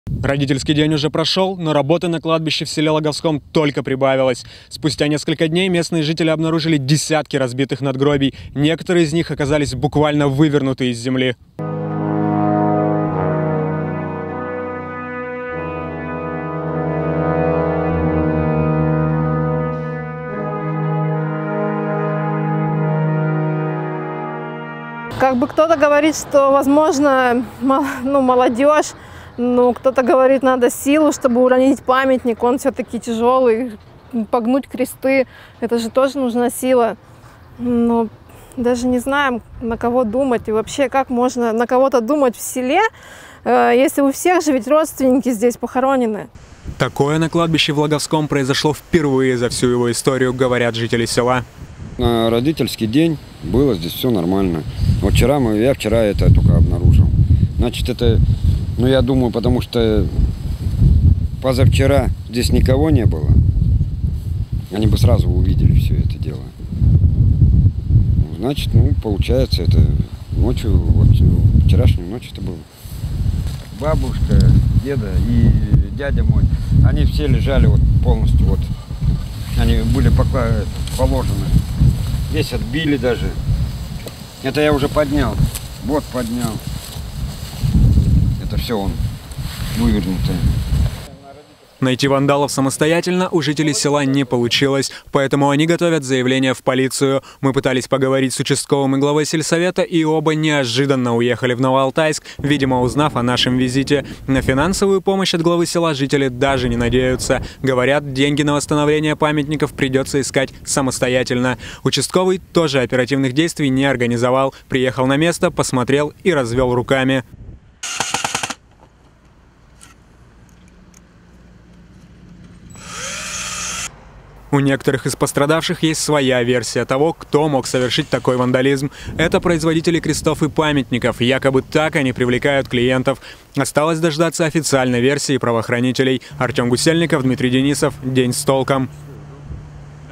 Вандалы разрушили более 60 надгробий на кладбище в Алтайском крае
В первомайском районе от рук вандалов пострадало сельское кладбище. Гранитные памятники разрушены, деревянные кресты вырваны из земли, а металлические – погнуты. Всего пострадало более 60 надгробий. На месте побывали наши корреспонденты.